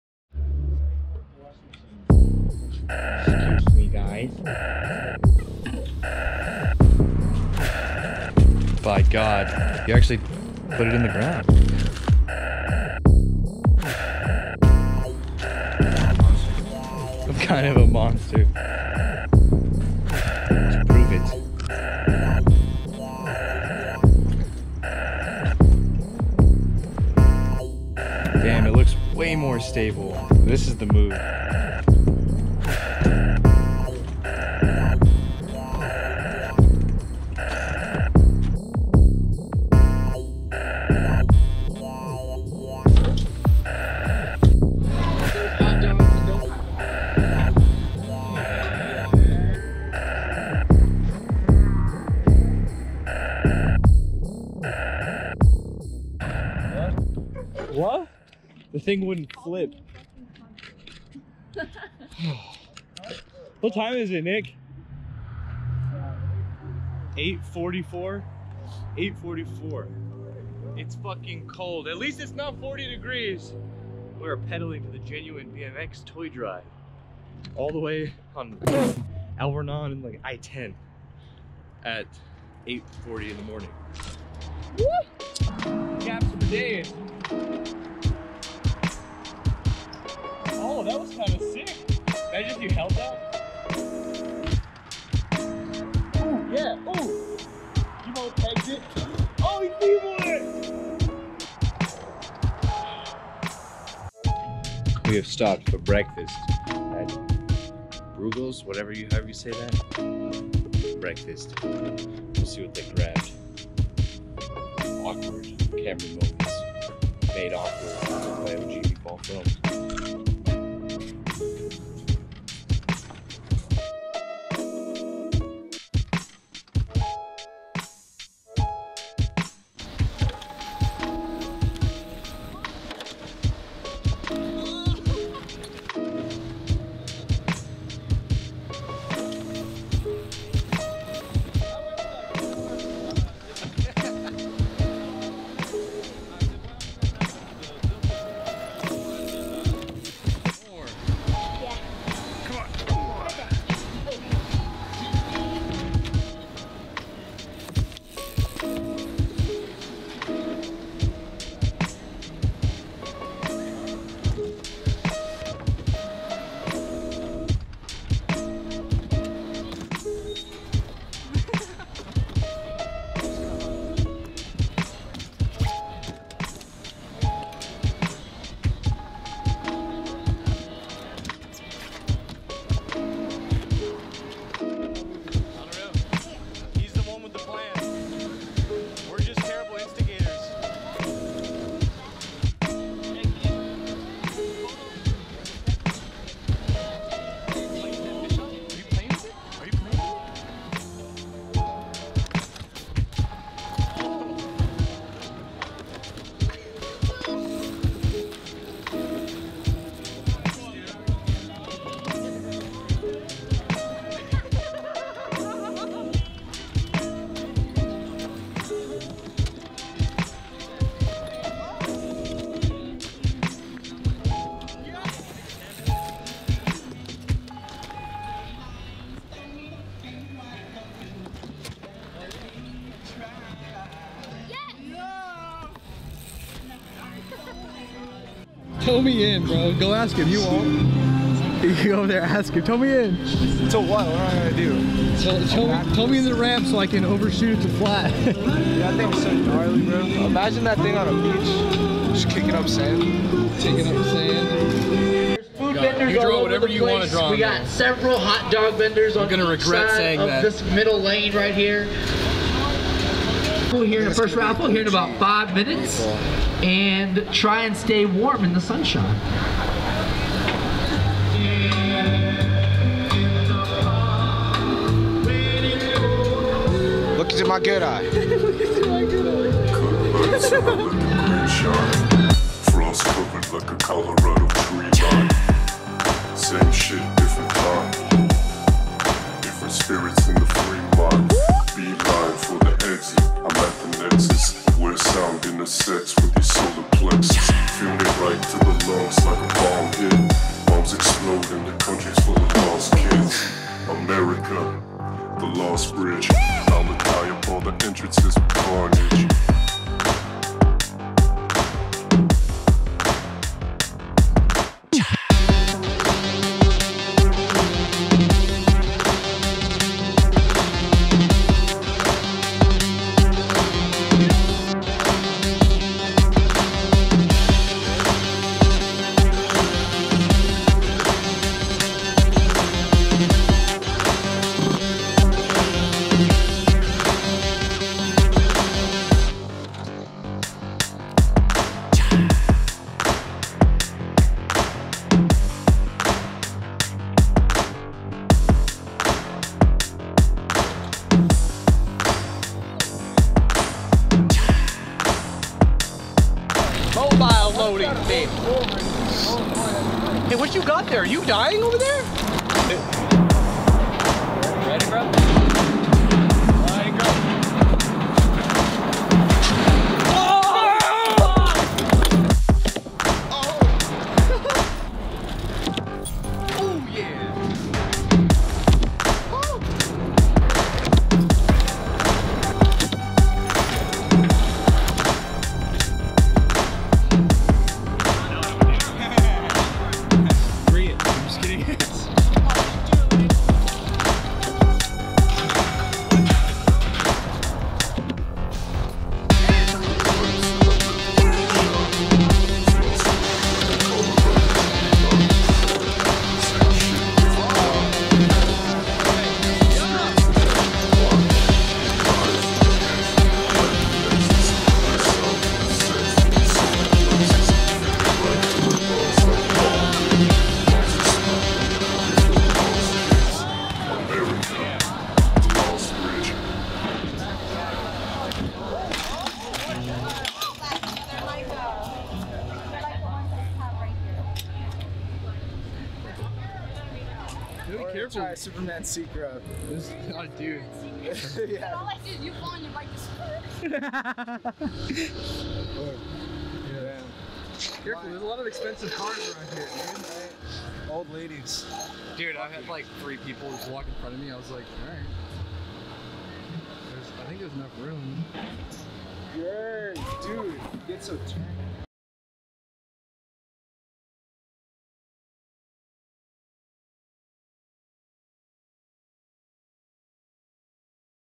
Woofits BMX Crew Vlog Ep.2 - Tucson BMX Jam (Feat. Genuine BMX, Adler Cromer and Locals)
In this episode, the crew pedals a cool 7 miles to meet up with Saul from Genuine BMX to ride their dope airbag set up. Of course we weren't the only ones there, the local scene rolled through and destroyed the ramps with their skills. Love you all, another dope Vlog. Untill we ride agaim

This Video contained Original Music by Xunknown Artist
Shout out to you bro. I'm also gonna re upload this with an edited version to credit everyone visually. I didn't have time the firs go around HAHAHAHA.